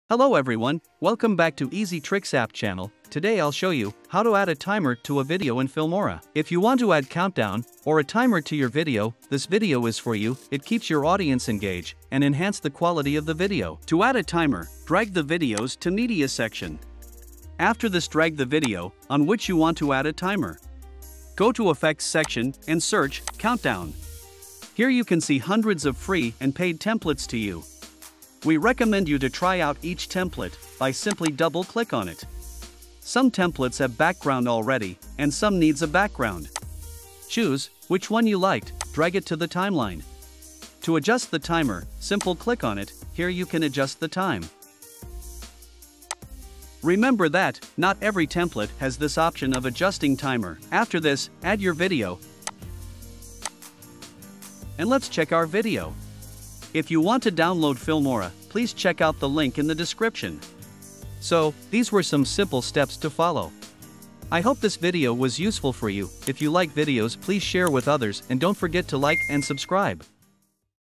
How To ADD A TIMER To A Video Filmora (EASY Tutorial)
Have you ever wanted to add a timer to your video projects? Look no further! This video will show you exactly how to do it in Filmora. We'll walk through the simple process of setting up a timer within the editing suite, from choosing which type of timer you want, customizing its look and feel, and then adding it to your project. Learn all this and more as we take you step-by-step through creating great-looking timers for your videos.